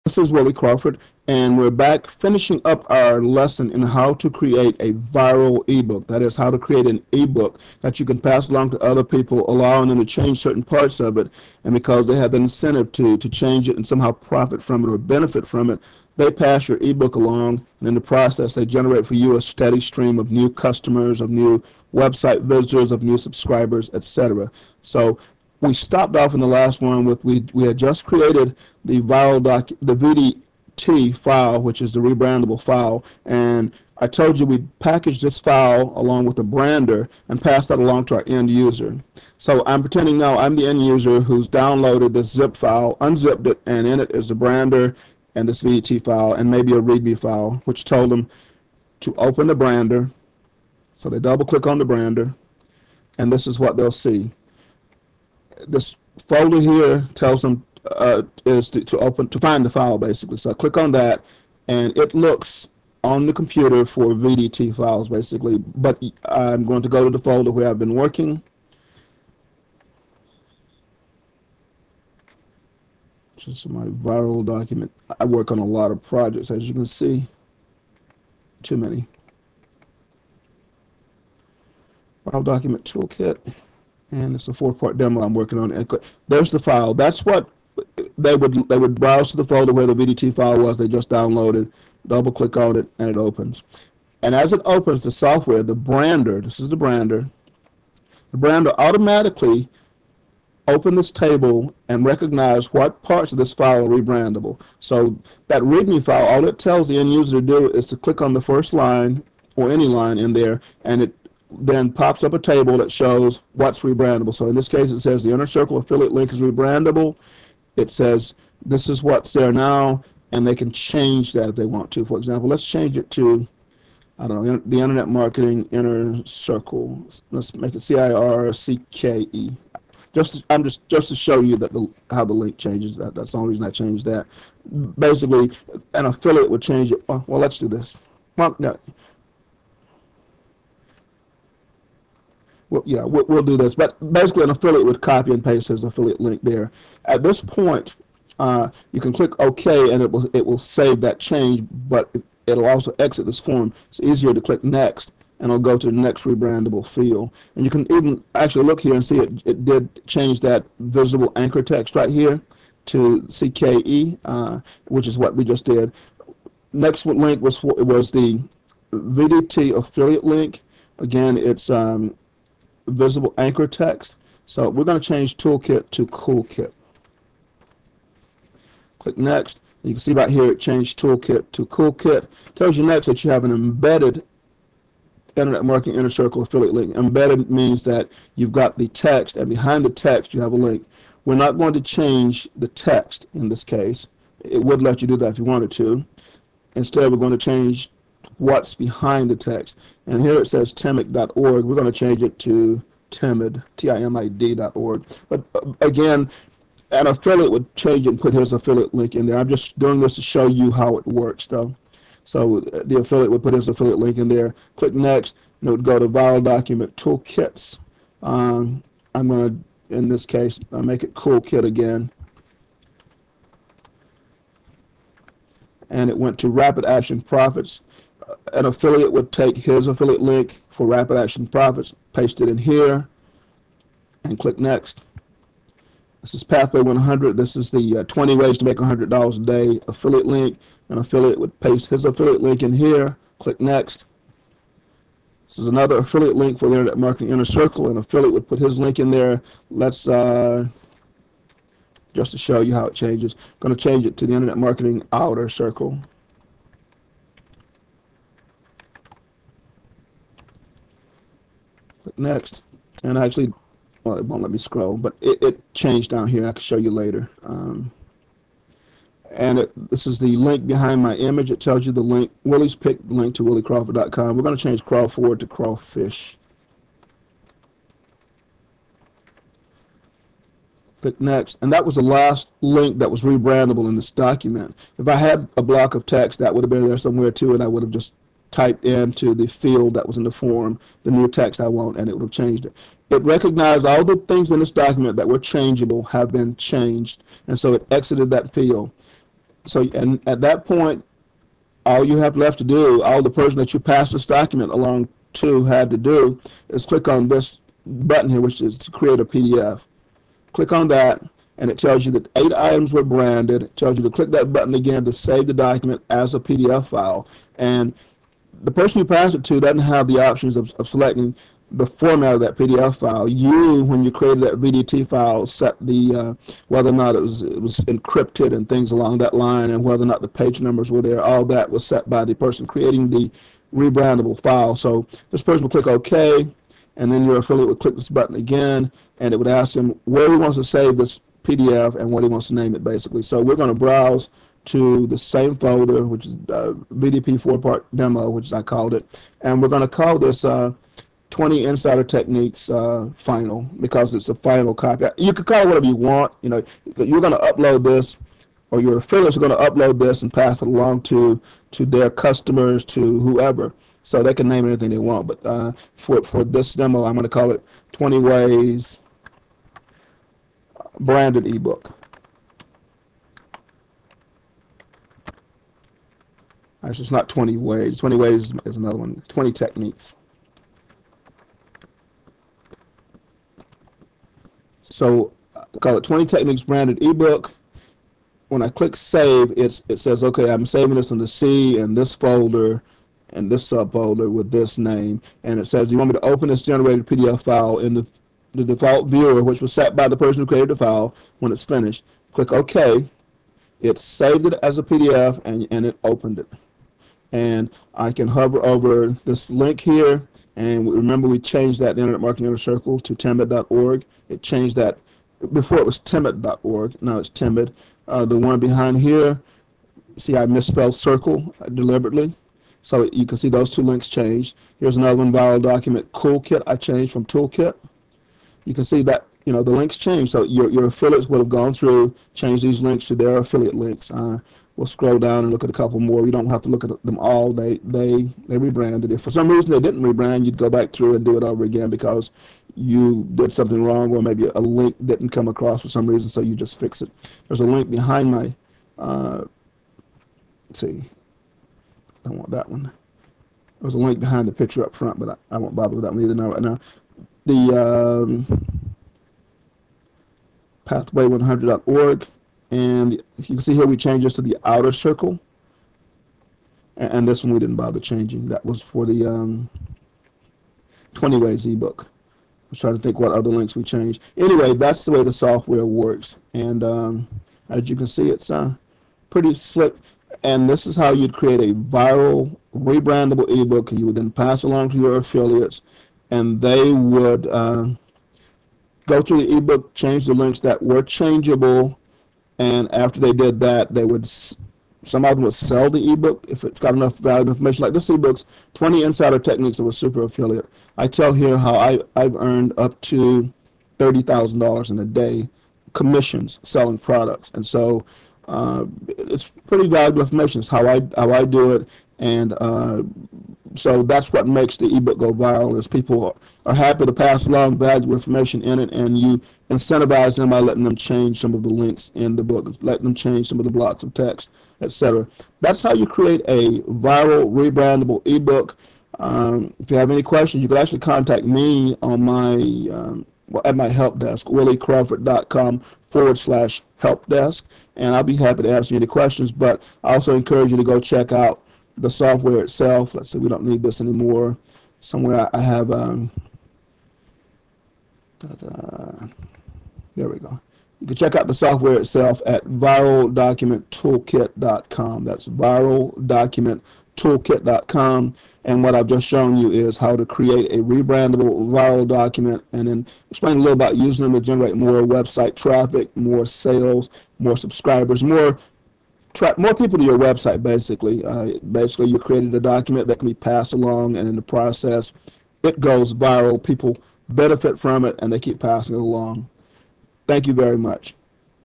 Create a Viral eBook Part 4
How to create a viral ebook from any document; a
rebrandable pdf has never been this easy. Anybody
can create a viral document in minutes with
Viral Document Toolkit.